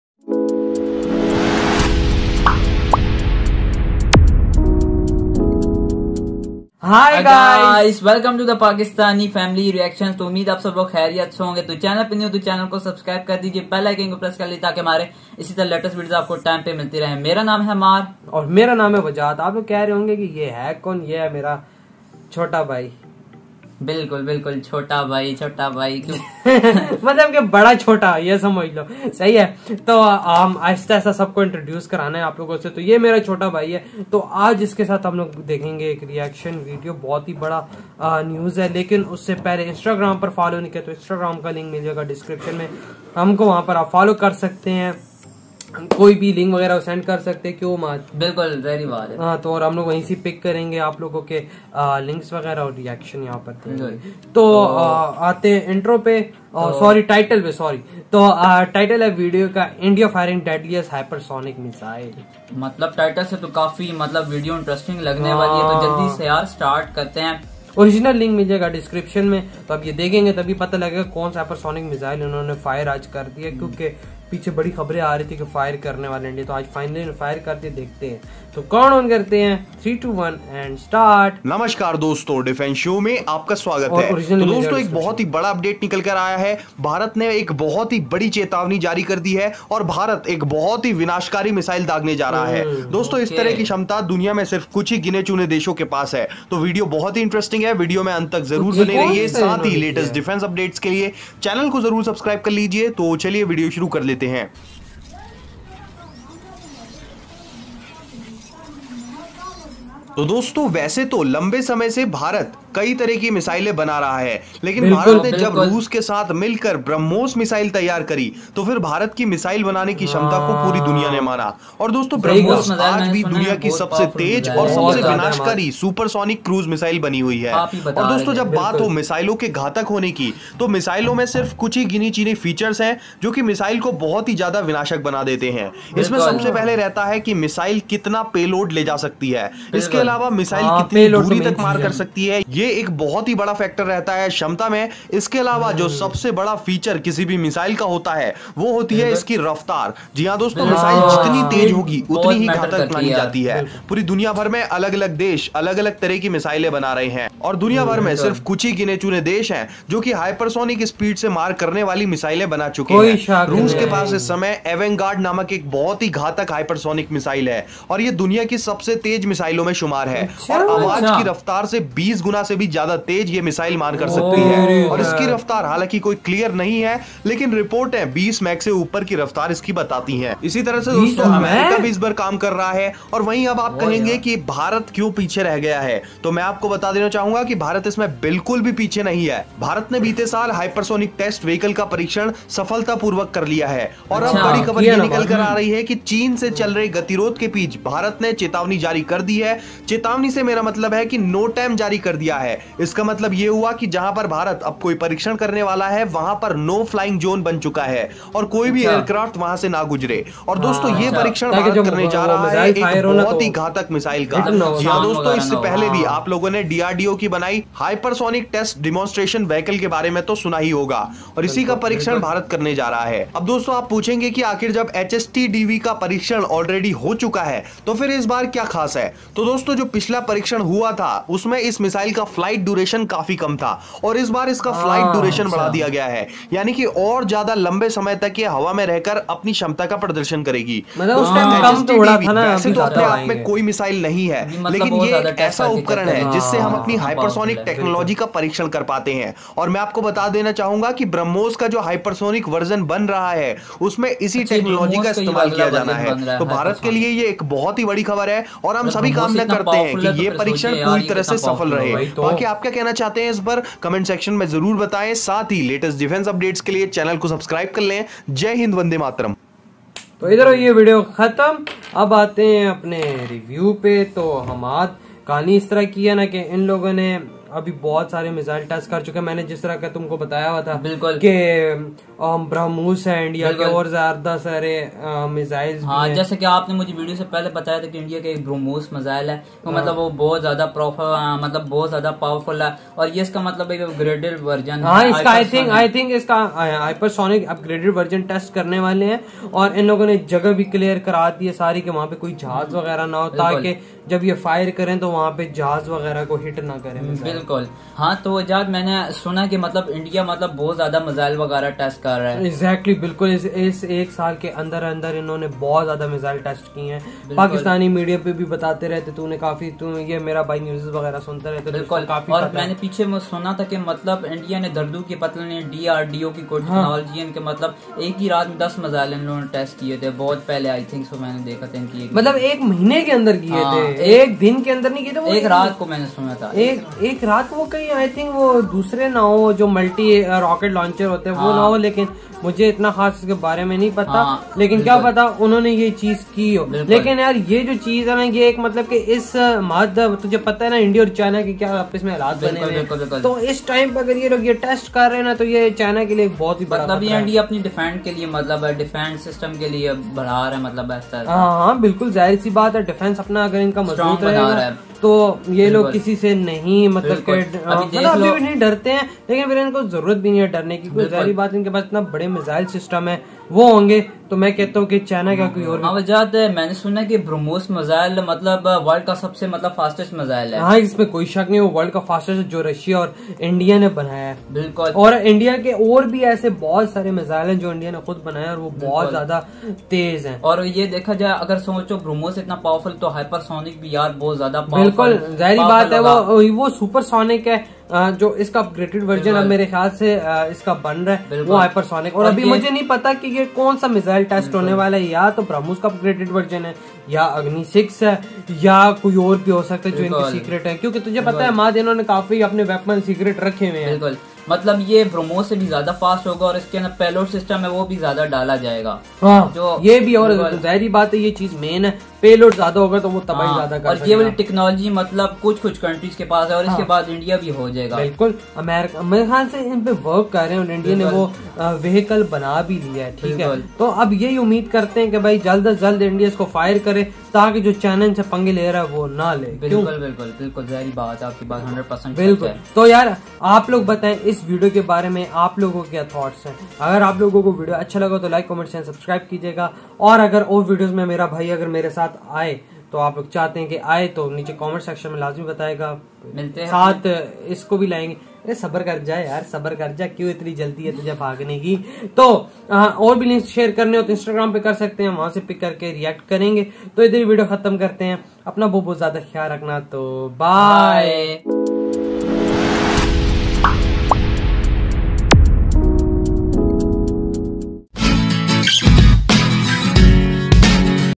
India Firing Today Deadliest Hypersonic Missile Shocking Reaction By|Pakistani Family Reacts
.credits.

     Wajahat:-    Sheikhwajahat007